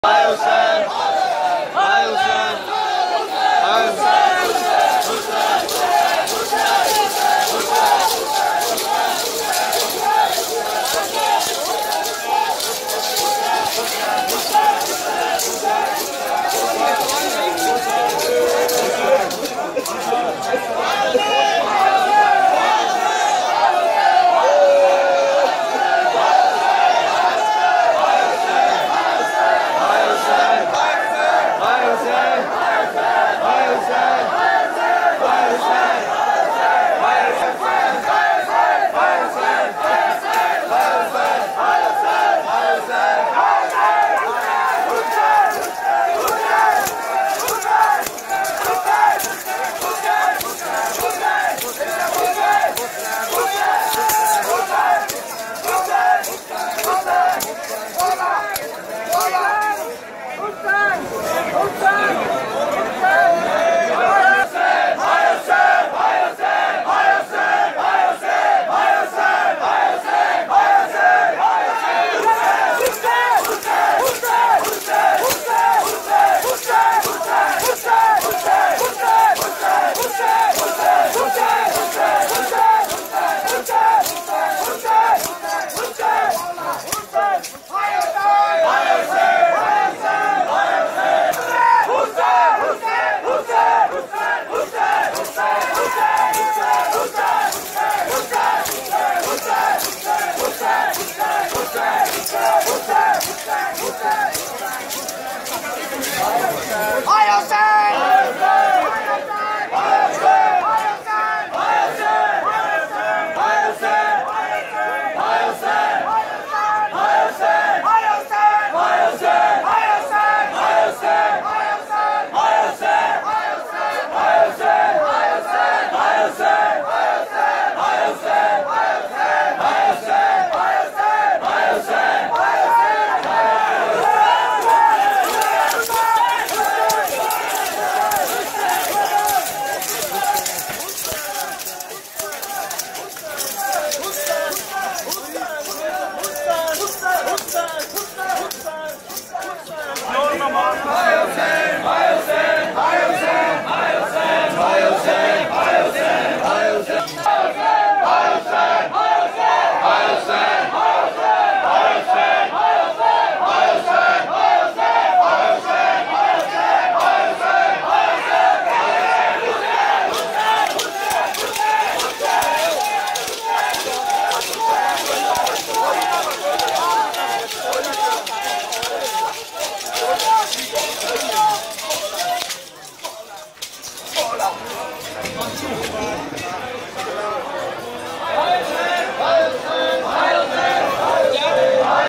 10th Moharram 2016 Zanjeer zani /Markazi Imam Bargah Gujarkhan
Syed Muqaddar Hussain a.s & his sons Kazmi Brothers